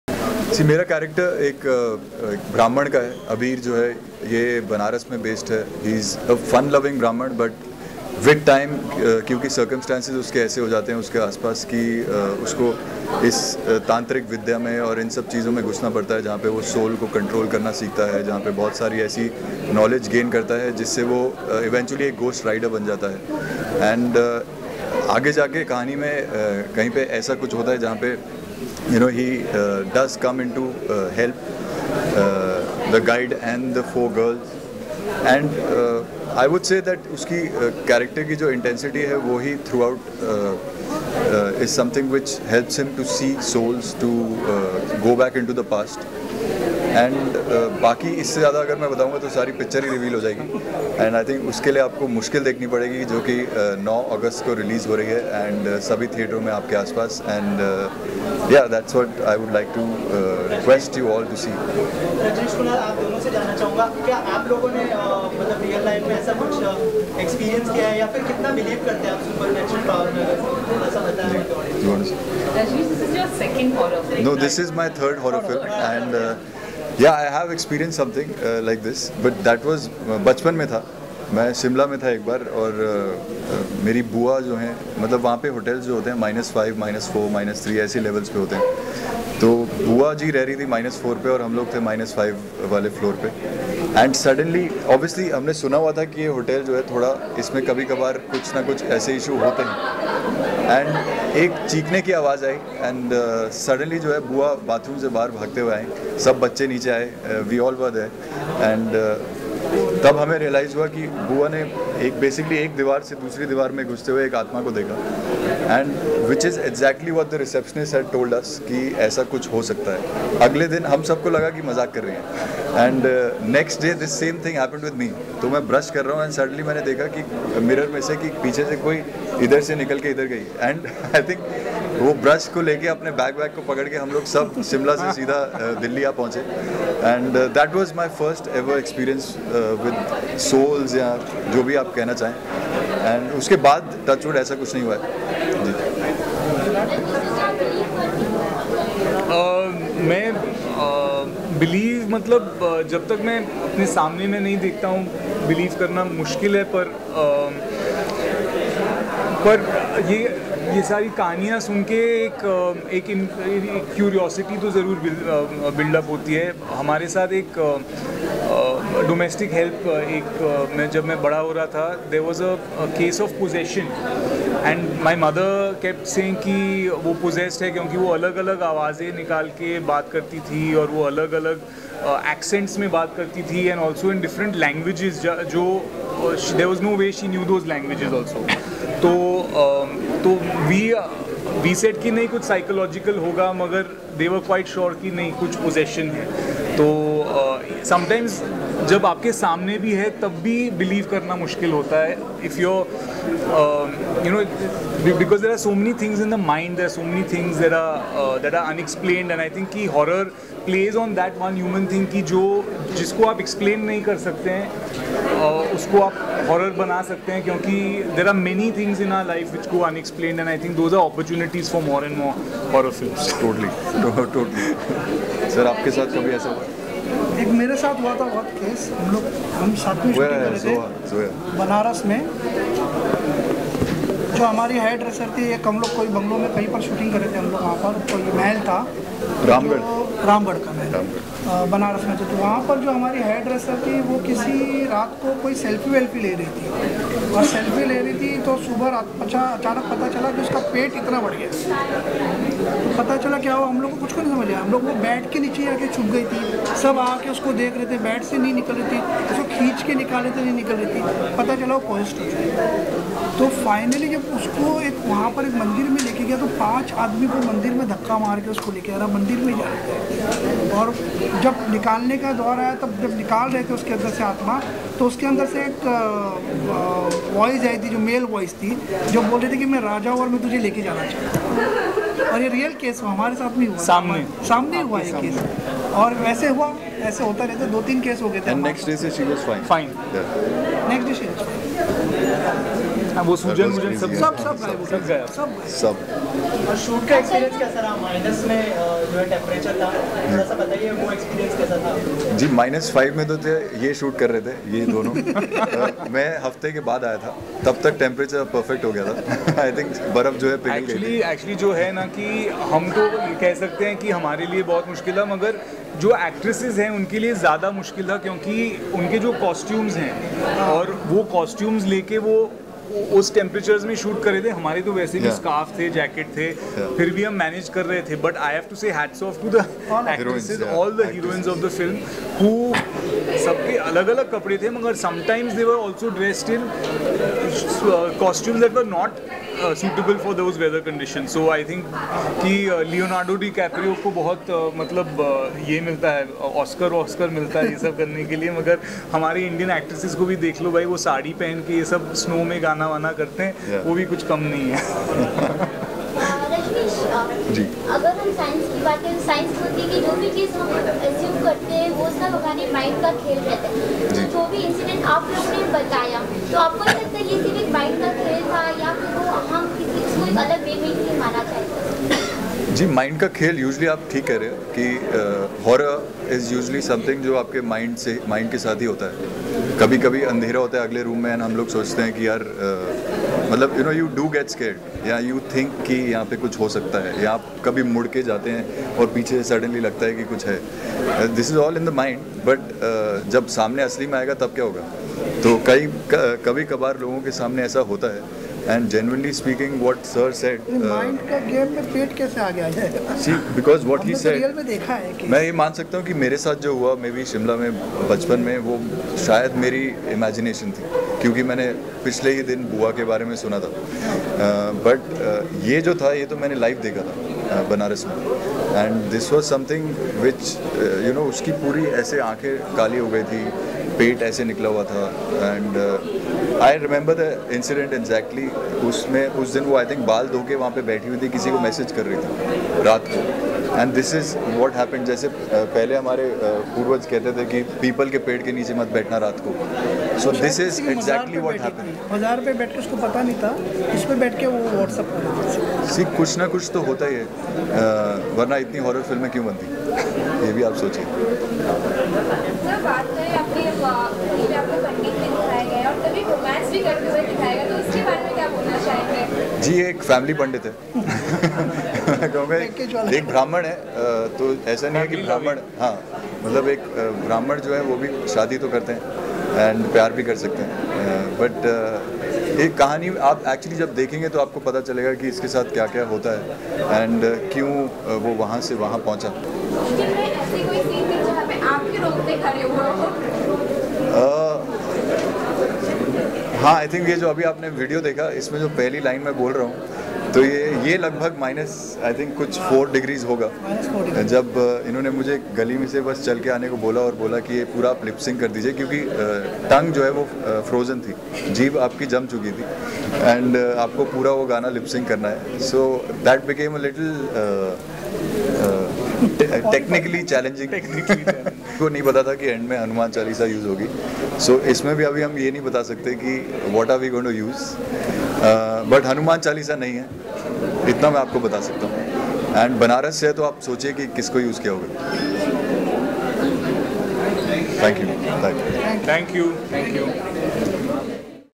Interview with Rajneesh Duggal & Rajiv S. Ruia | Mushkil | Bollywood Chronicle
Interview with Rajneesh Duggal & Rajiv S. Ruia